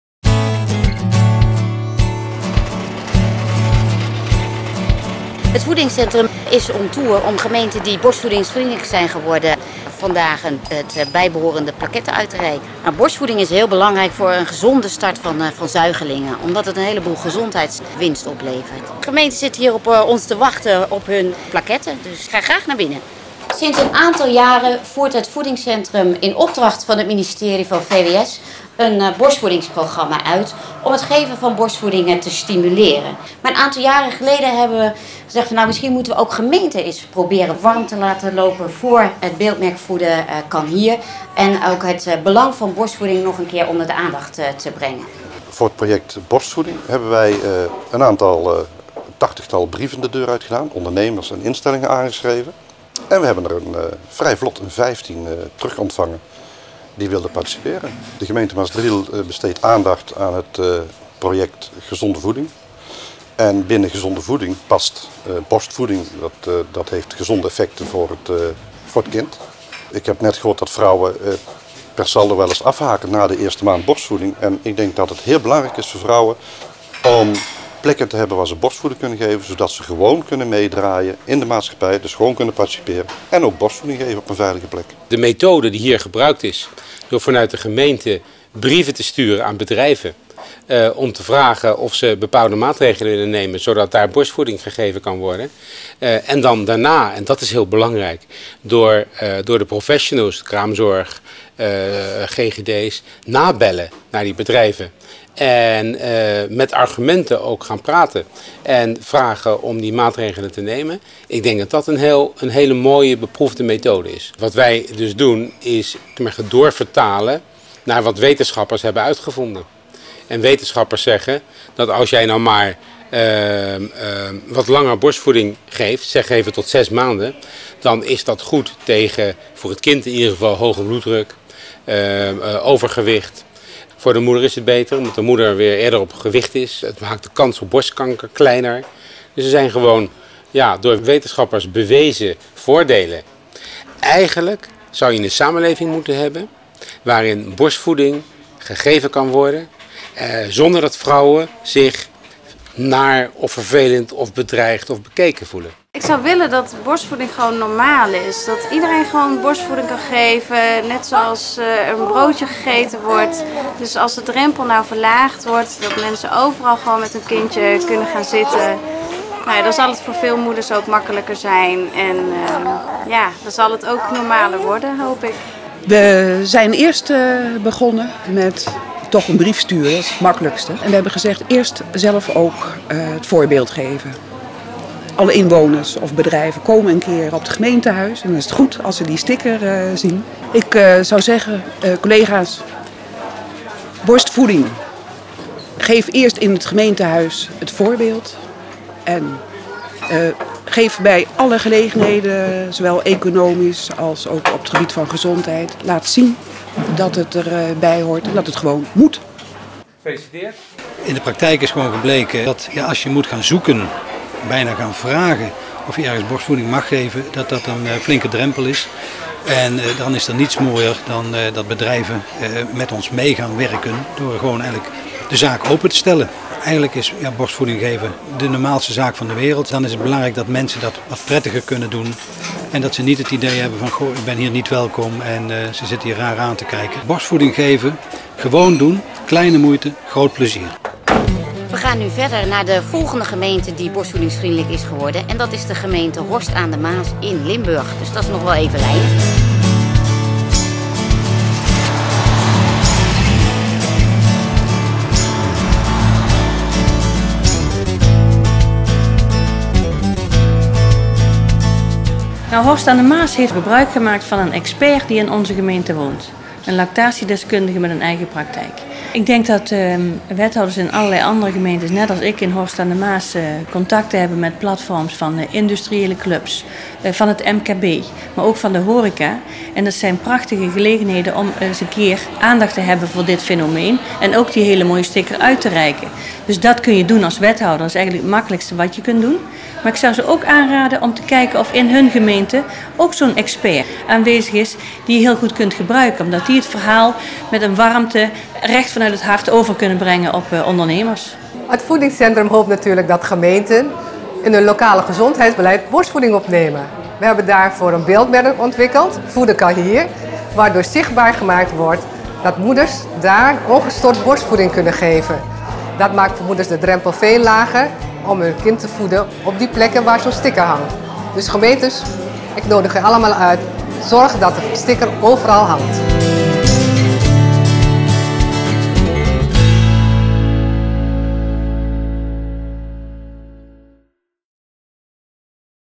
Uitreiking plaquettes "Borstvoedingsvriendelijke gemeenten" | Voedingscentrum
De drempel verlagen voor jonge moeders om hun kind in het openbaar te voeden. Dat hebben gemeenten Horst aan de Maas, Tiel, Maasdriel en Geldermalsen gedaan. Zij zijn sinds 2011 'Borstvoedingsvriendelijke Gemeente'. Ze hebben onder andere het gebruik van het beeldmerk 'Voeden kan hier' gestimuleerd.

Wil je meer weten over gezonde en duurzame voeding? Bezoek onze website en volg ons op social media!